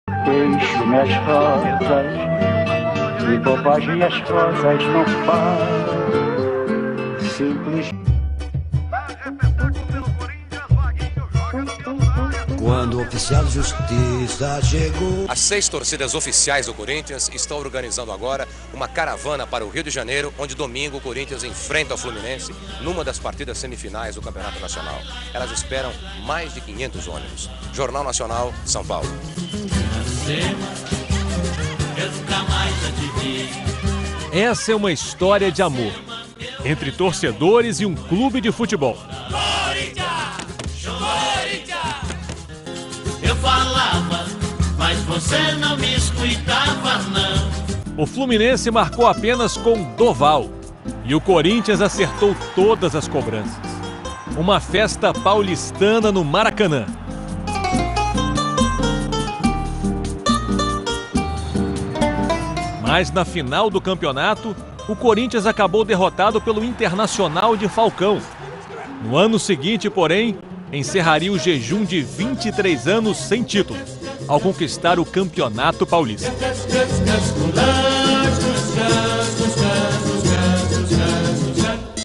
História do Corinthians 1976 e 1977
Da invasão corintiana no Maracanã ao título de 1977. Imagens TV Globo